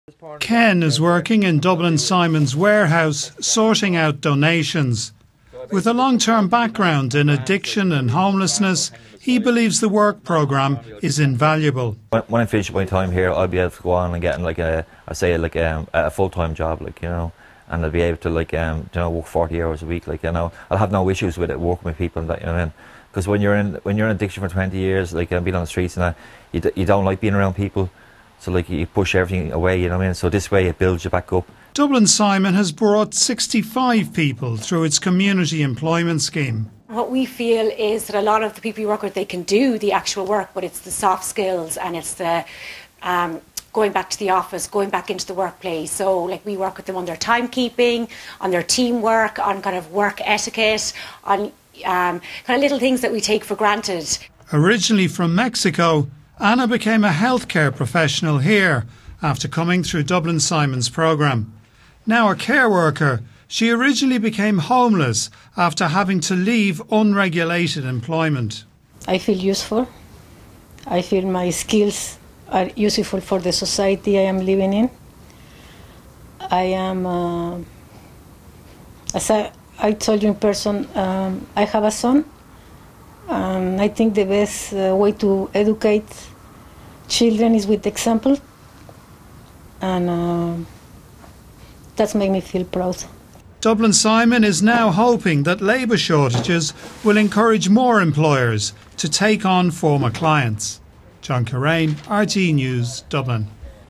Charity hopes scheme will help more homeless people get jobs
Dublin Simon is hoping to expand its work experience programme for homeless people.

It wants more employers in the private sector to consider hiring its trainees.